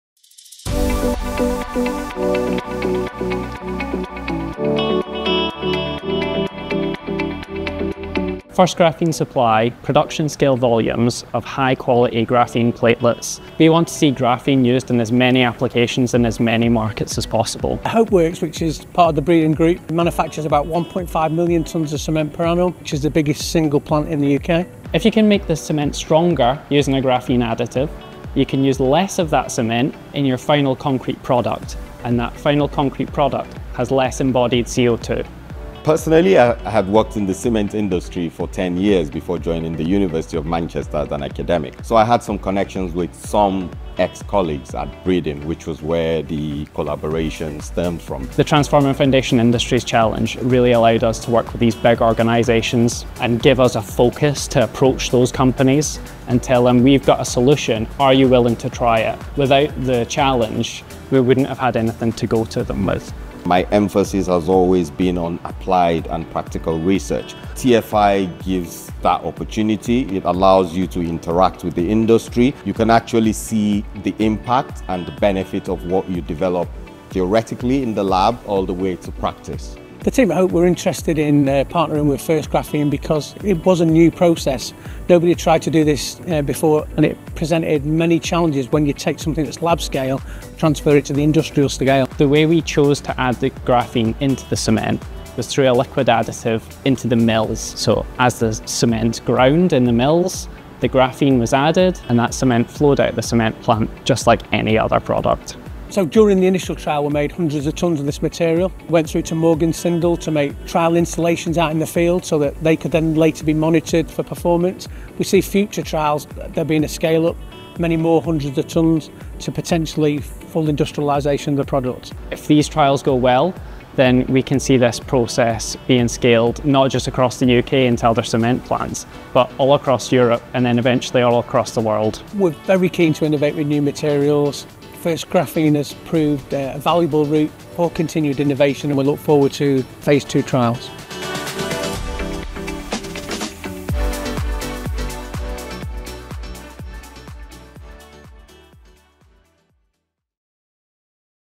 World-leading trials of graphene-enhanced cement - delivering industry changing results
As an industry-leading producer and supplier of PureGRAPH® - a high-performing graphene product - we pride ourselves on providing extensive benefits to industries around the world.

In the UK, we have worked with the cement and concrete sector to realise these benefits through world-leading trials of graphene-enhanced cement.

Find out more about our partnership with Breedon Group, Morgan Sindall Infrastructure, and the University of Manchester.